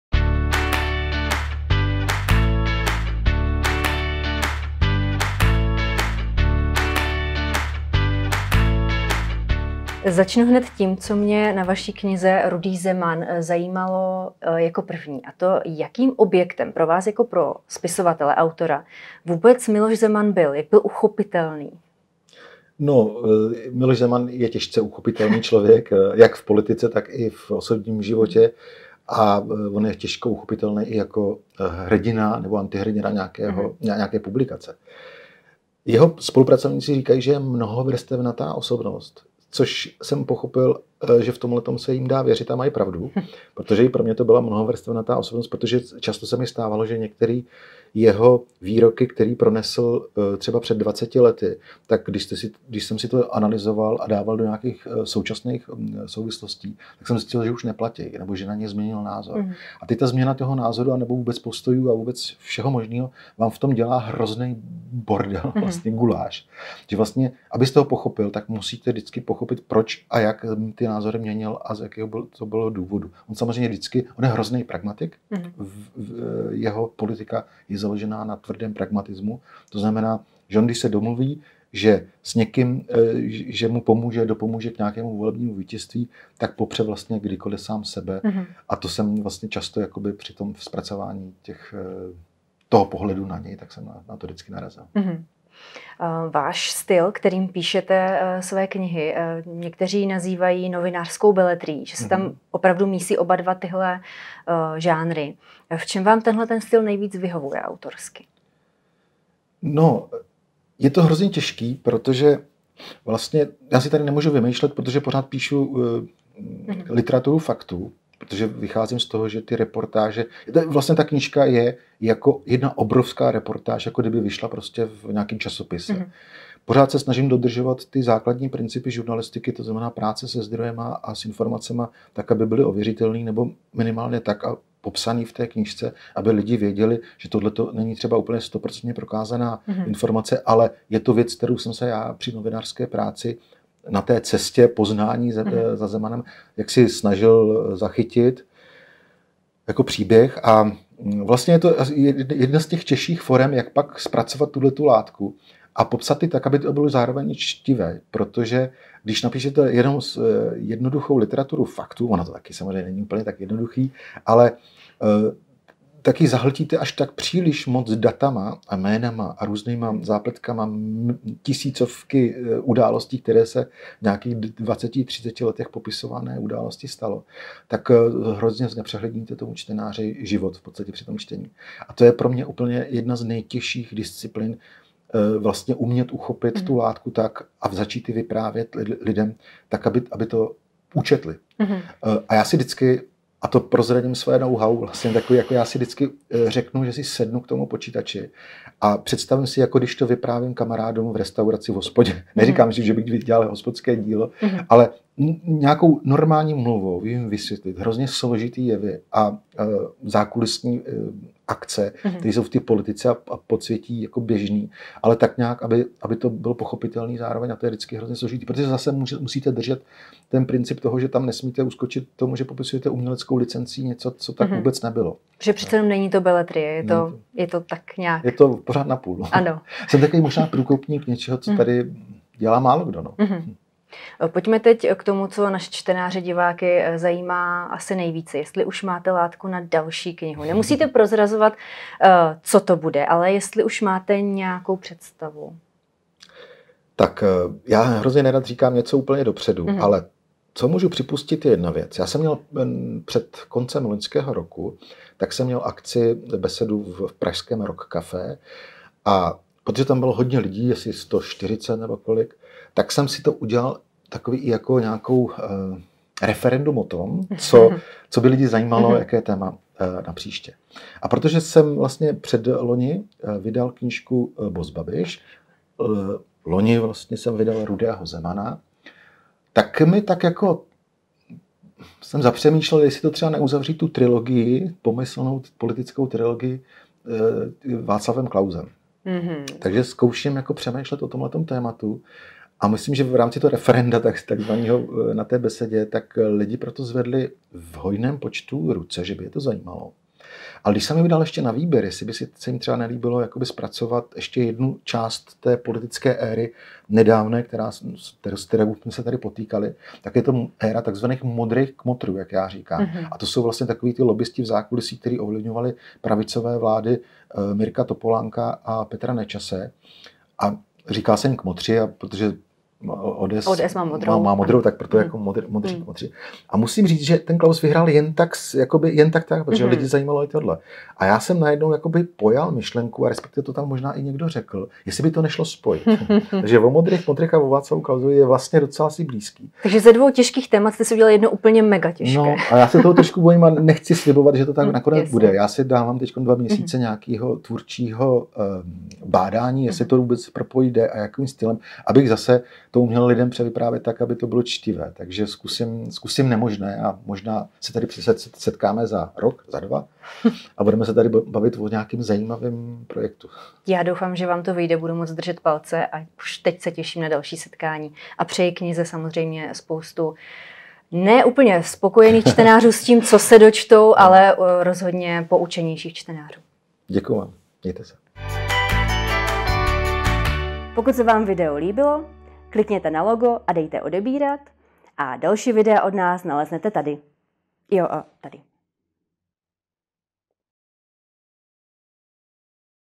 Jaroslav Kmenta - Rudý Zeman
Investigativní novinář a spisovatel Jaroslav Kmenta představuje svou novou knihu Rudý Zeman, která se mimo jiné zaobírá také okolnostmi mocenského paktu s oligarchou Andrejem Babišem. Kniha Rudý Zeman tak volně navazuje na bestseller Boss Babiš z roku 2017.
Co je pro Jaroslava Kmentu na psaní "novinářské beletrie" nejtěžší a jak se mu pracovalo s pragmatickou osobností Miloše Zemana se dozvíte v našem rozhovoru.